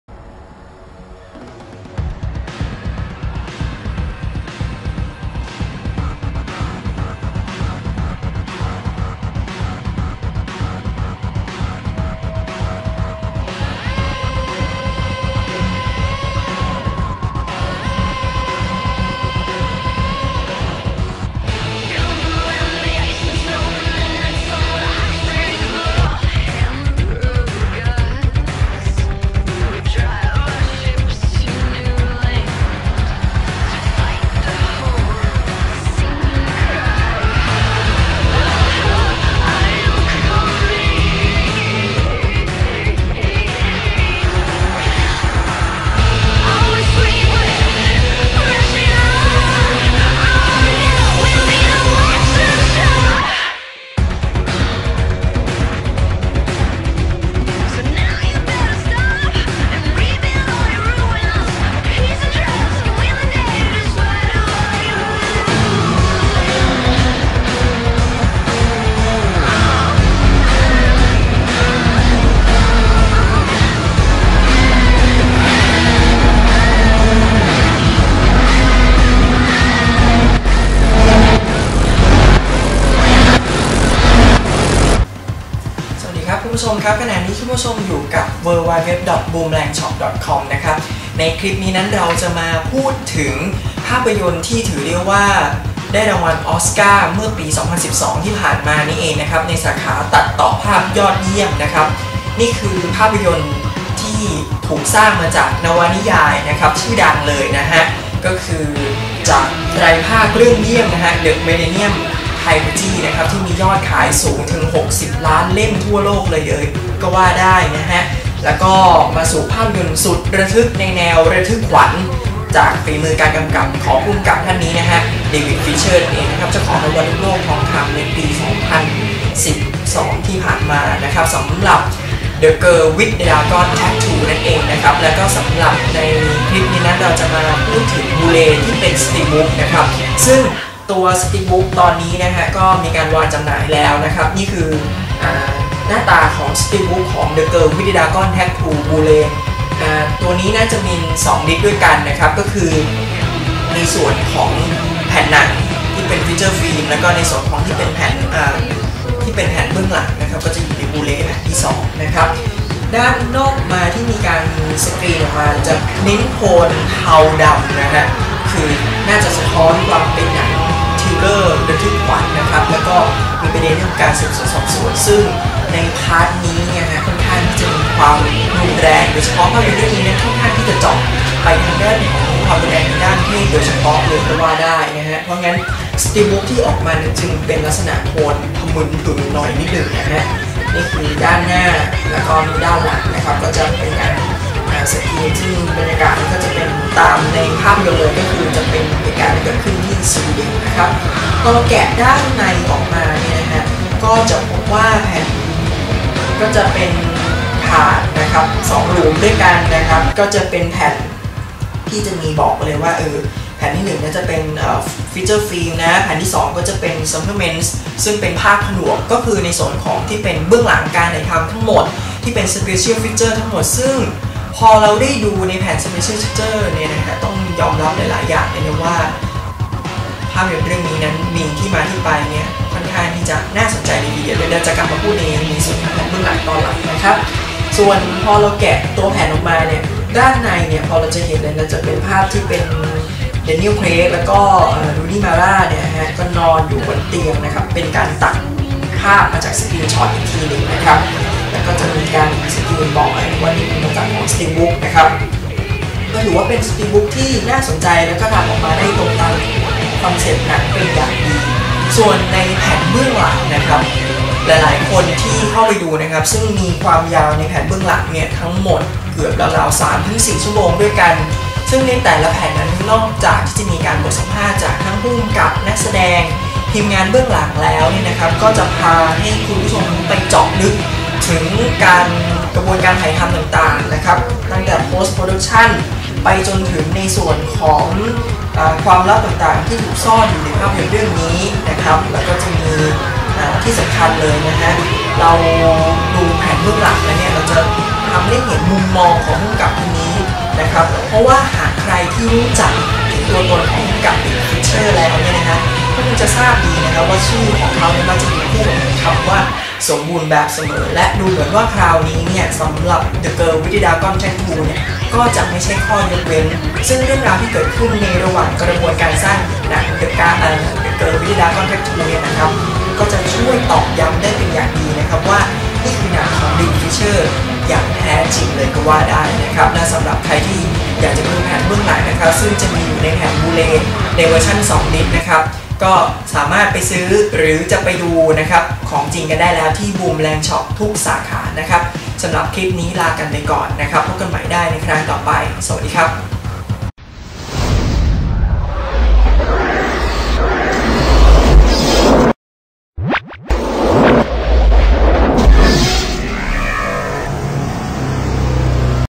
The Girl With The Dragon Tattoo BD 2 Disc Steelbook
ชนะเลิศออสการ์ 2012 สาขาตัดต่อภาพยอดเยี่ยม จากนวนิยายไตรภาคเรื่องเยี่ยม The Millennium Trilogy ที่มียอดขายสูงถึง 60 ล้านเล่มใน 46 ประเทศทั่วโลก มาสู่ภาพยนตร์สุดระทึกด้วยฝีมือการกำกับของ เดวิด ฟินเชอร์ เจ้าของรางวัลลูกโลกทองคำปี 2012

# Link Product The Girl With The Dragon Tattoo BD 2 Disc Steelbook